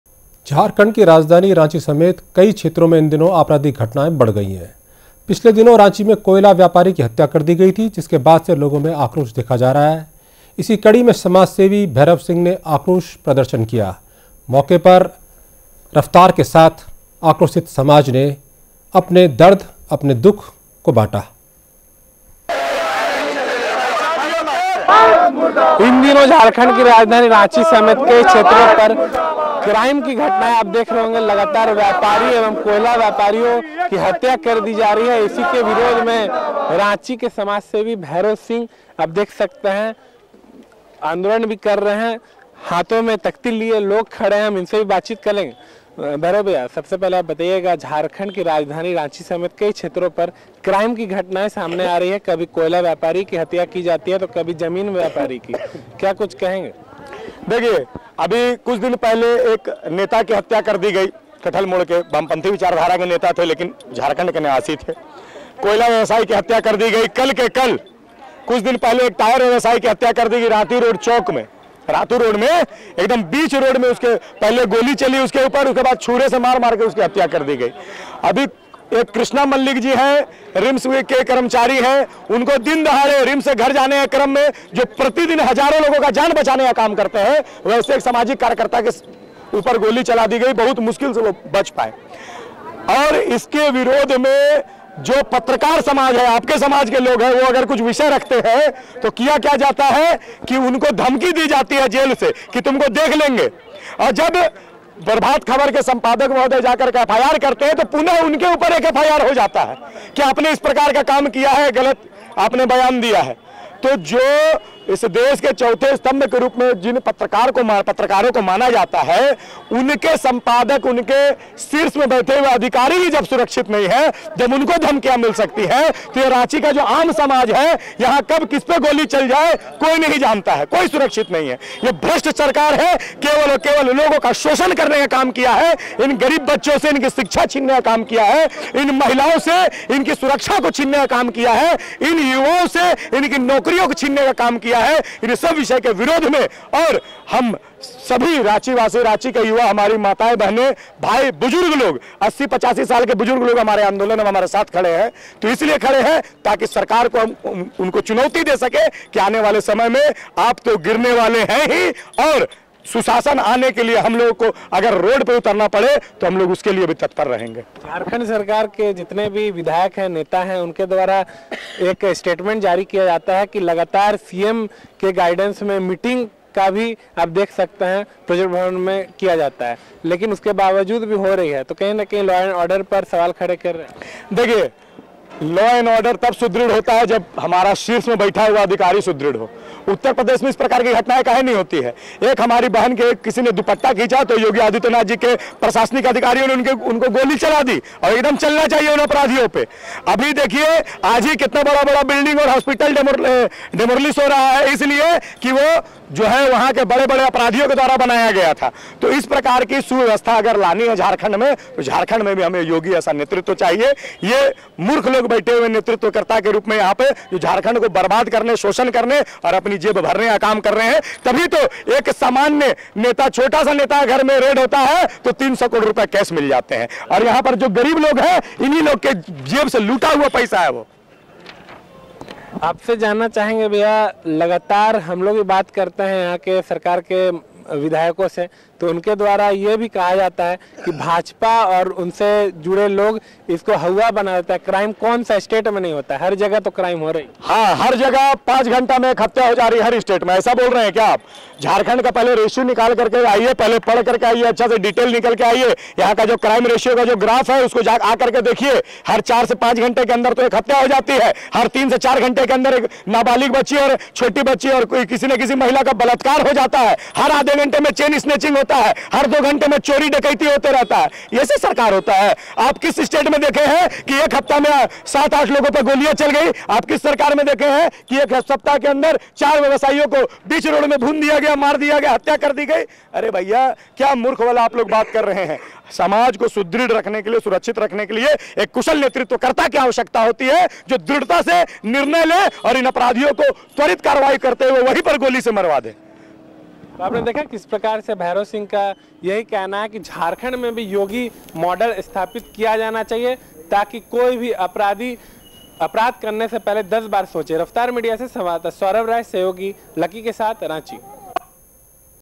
रांची : राज्य में बढ़ती अपराध को लेकर, भैरव सिंह का आक्रोश प्रदर्शन
Raftaar Media